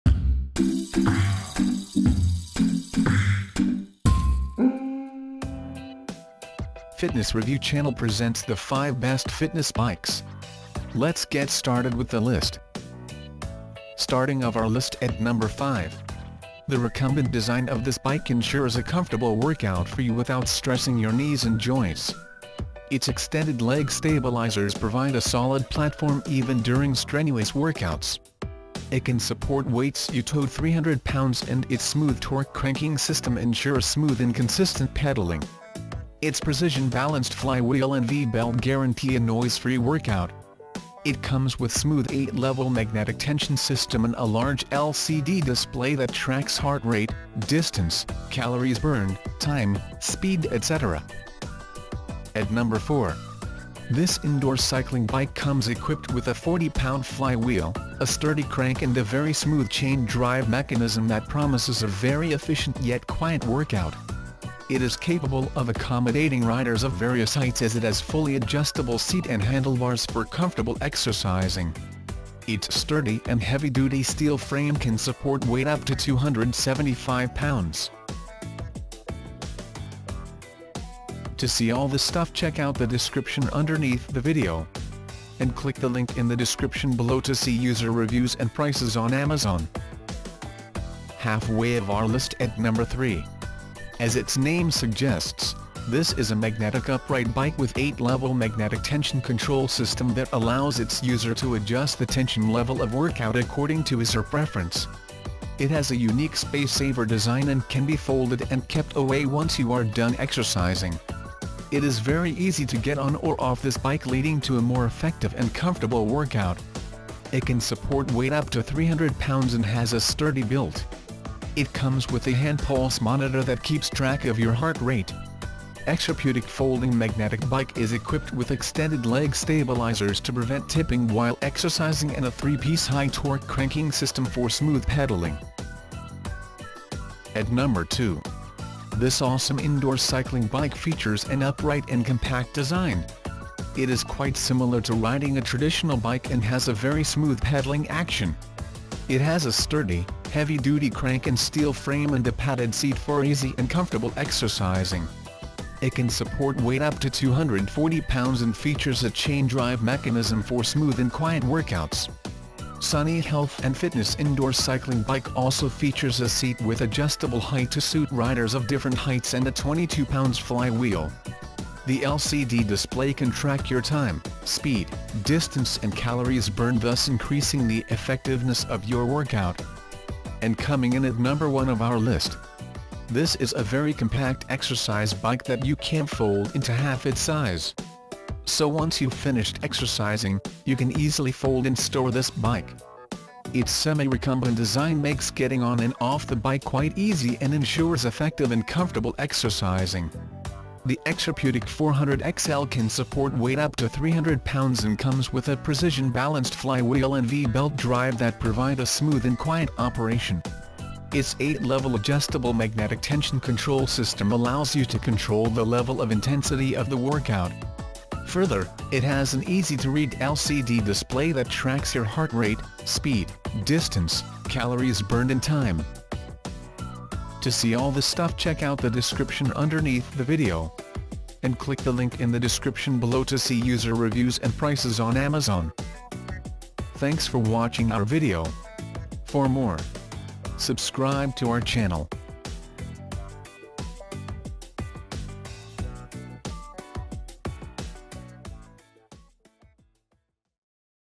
Review: Best Fitness Bikes 2018
Are you looking for the Best Fitness Bikes. We spent hours to find out the Best Fitness Bikes for you and create a review video. In this video review you will find the top products list, what is the feature of these products and why you should buy it.